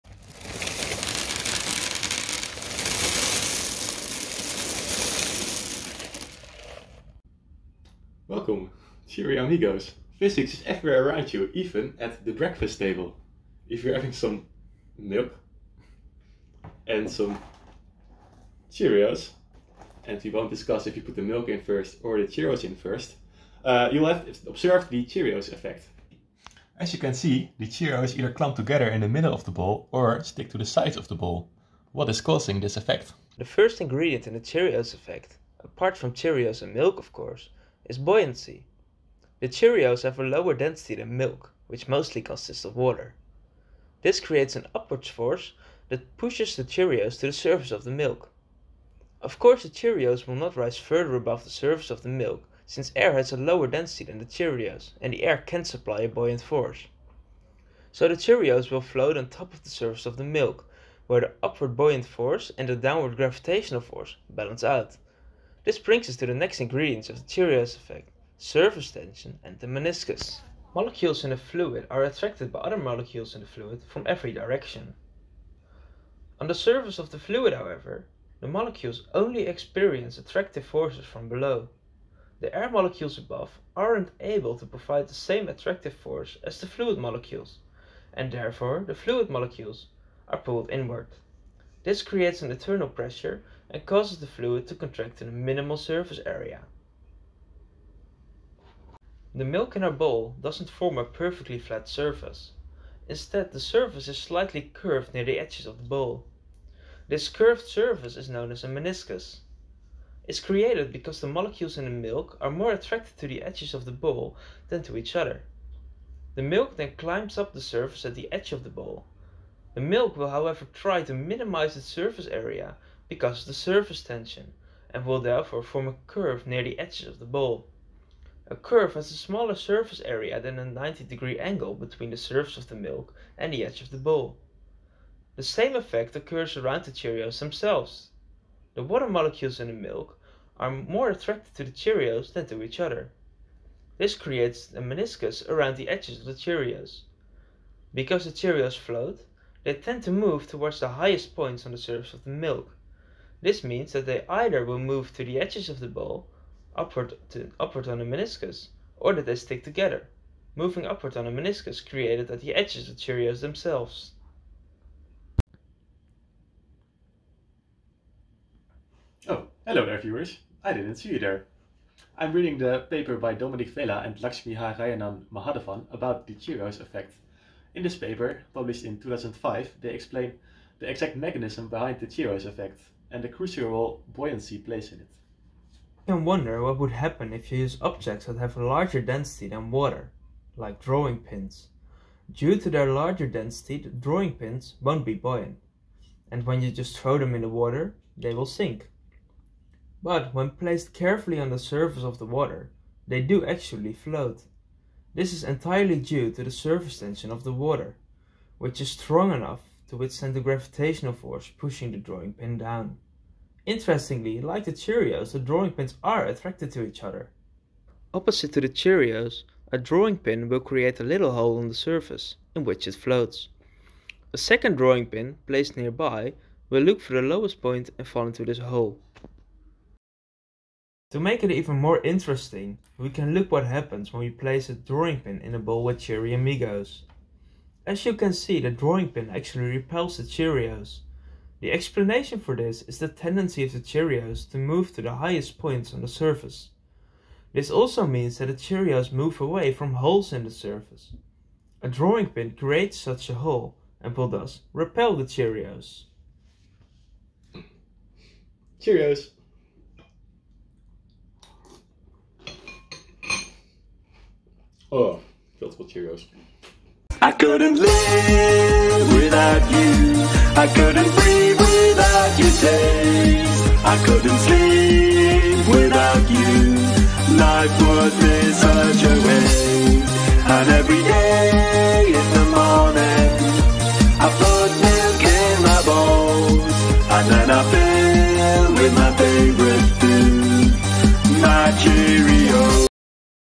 The Cheerios Effect
This video explains the Cheerios Effect. The Cheerios Effect is the phenomenon in which Cheerios
 floating in a bowl of milk stick to the edges of the bowl or to each-other.